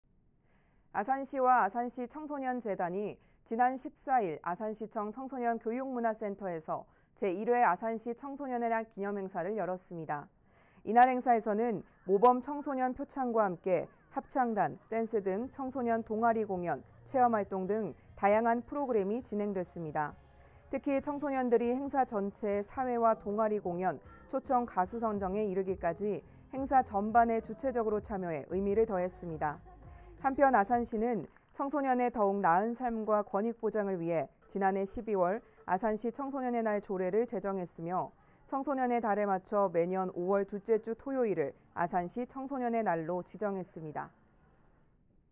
[B tv 중부뉴스]아산시, ‘제1회 아산시 청소년의 날’ 기념행사 성료
아산시와 아산시청소년재단이 지난 14일
아산시청 청소년교육문화센터에서
제1회 아산시 청소년의 날 기념행사를 열었습니다.

이날 행사에서는 모범 청소년 표창과 함께
합창단, 댄스 등 청소년 동아리공연, 체험 활동 등
다양한 프로그램이 진행됐습니다.

특히, 청소년들이 행사 전체의 사회, 동아리 공연,  초청 가수
선정에 이르기까지 행사 전반에 주체적으로 참여해 의미를 더했습니다.

한편 아산시는 청소년의 더욱 나은 삶과 권익 보장을 위해
지난해 12월 ‘아산시 청소년의 날’조례를 제정했으며
청소년의 달에 맞춰 매년 5월 둘째 주 토요일을
‘아산시 청소년의 날’로 지정했습니다.